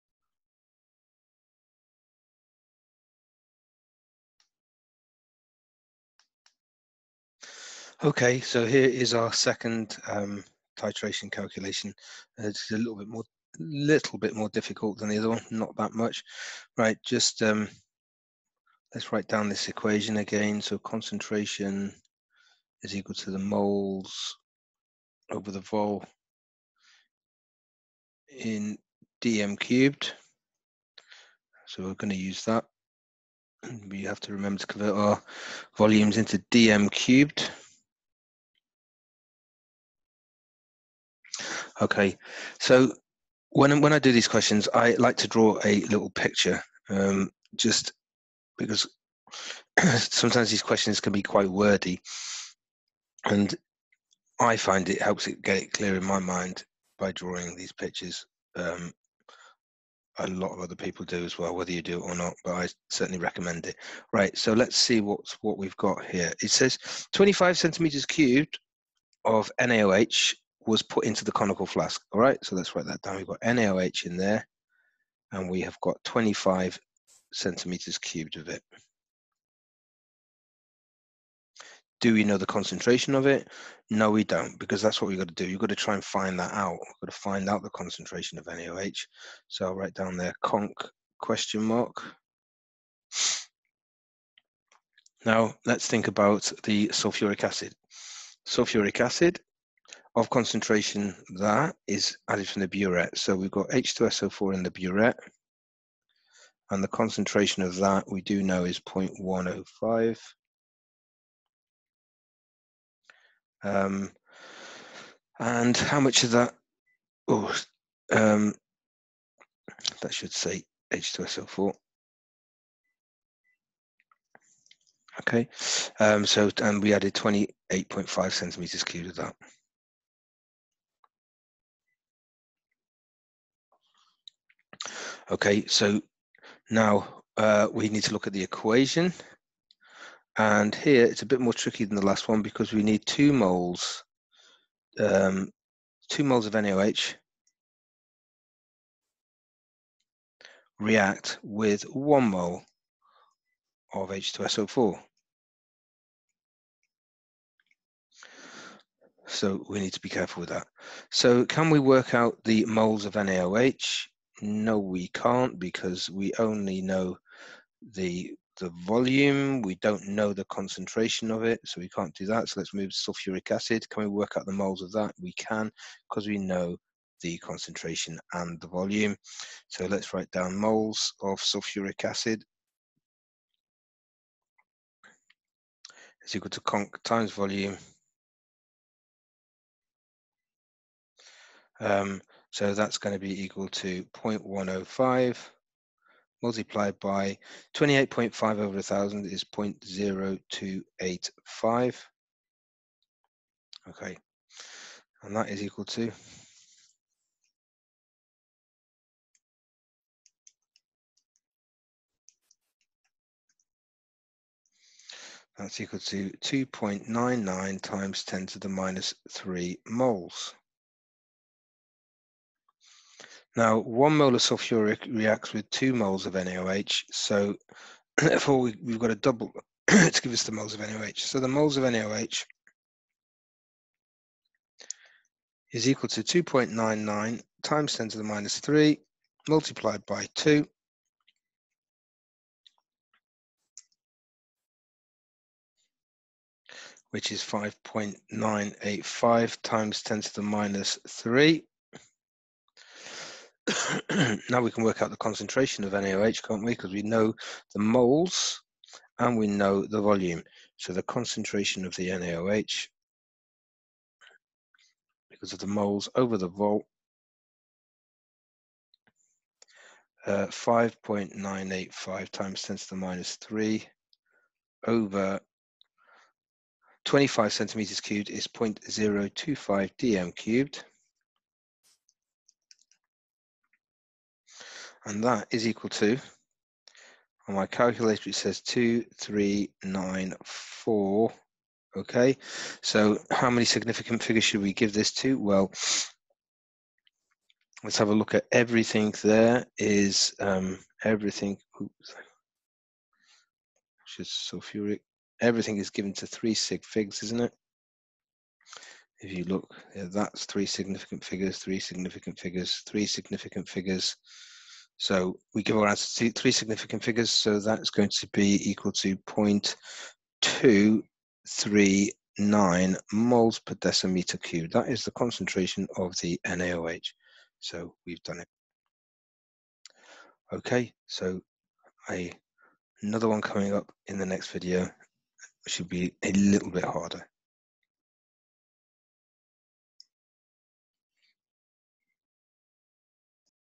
1.2 Amount of Substance No 2 Titration (easy)
AQA A-level Chemistry 1.2 Amount of Substance
Titration calculation- a bit harder than GCSE but easy for A-level